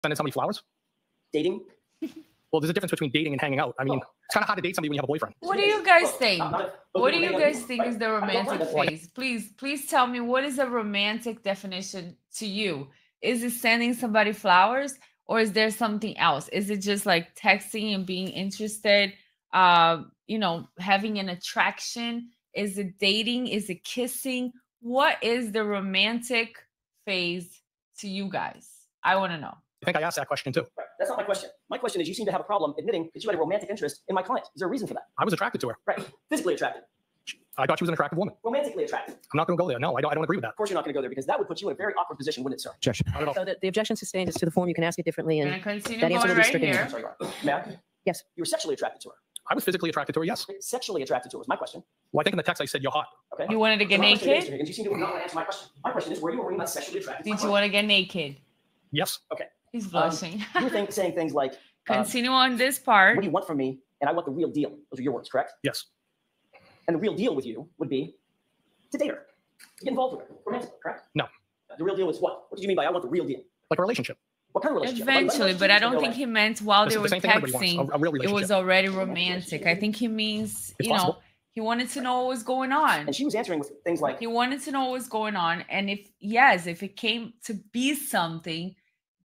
Is Texting the New Dating Courtroom Drama Unfolds!
Mr. Higgins discusses the complexities of romantic interest in a courtroom setting while analyzing text messages.
Sending somebody flowers, dating? well, there's a difference between dating and hanging out. i mean it's kind of hard to date somebody when you have a boyfriend. what do you guys think? what do you guys think is the romantic phase? please please tell me what is a romantic definition to you? is it sending somebody flowers or is there something else? is it just like texting and being interested? uh, you know, having an attraction, is it dating? is it kissing? what is the romantic phase to you guys? i want to know. i think i asked that question too. That's not my question. My question is: you seem to have a problem admitting that you had a romantic interest in my client. Is there a reason for that? I was attracted to her Right Physically attracted. I thought she was an attractive woman, Romantically attracted. I'm not going to go there. No, I don't agree with that. Of course you're not going to go there, because that would put you in a very awkward position, wouldn't it, sir? The objection sustained is to the form. You can ask it differently and that answer will be stricken. Can I continue going right here? Ma'am, Yes, You were sexually attracted to her. I was physically attracted to her. yes, Sexually attracted to her was my question. Well, I think in the text I said you're hot. You wanted to get naked. You seem to not want to answer my question. My question is: were you or were you not sexually attracted to her? You need to want to get naked. yes, okay, he's blessing. you think. saying things like: continue on this part. what do you want from me and i want the real deal. those are your words, correct? yes, and the real deal with you would be to date her. get involved with her, correct? no, the real deal is what. what do you mean by? i want the real deal, like a relationship, what kind of relationship eventually? but i don't think he meant while they were texting it was already romantic. i think he means you know. he wanted to know what was going on and she was answering things like. he wanted to know what was going on and if yes, if it came to be something,